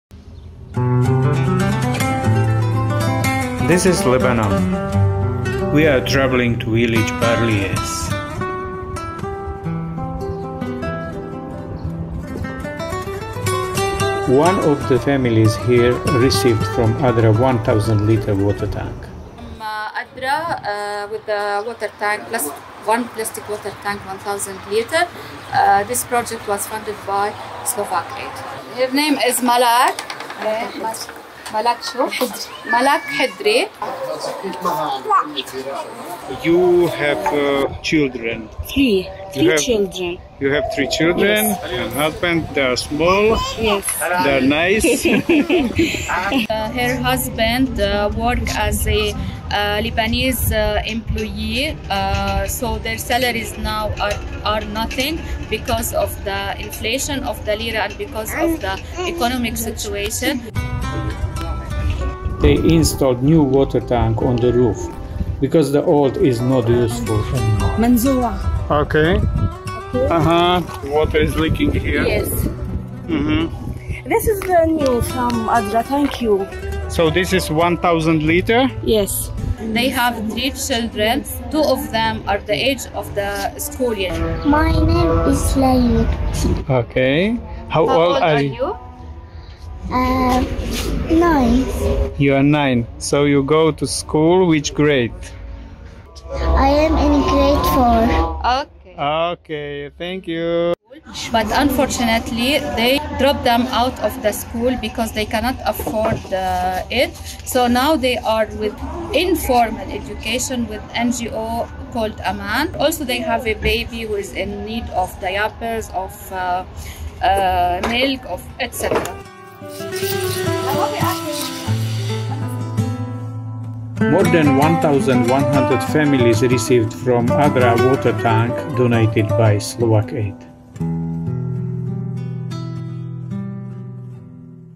Safe Water for Families in Lebanon's Bekaa Valley | ADRA Slovakia
The unavailability of safe potable water in Lebanon presents a challenge not only for Syrian refugees but also for the local Lebanese community. Many households are forced to utilize water stored in ponds, reservoirs, or water tanks that are not safe, leading to various health issues such as cholera, hepatitis A, and scabies.

That's why we decided to enhance the capacities of low-income households with limited access to water sources by providing them with 1,082 plastic tanks with a capacity of 1,000 liters each.

This initiative aimed to bolster the self-sufficiency of the vulnerable Lebanese community and Syrian refugee families in 6 locations in the Bekaa Valley. One of these families includes Malak's, whose tank guaranteed access to safe drinking water, enhancing their quality of life.

The project was financed by SlovakAid.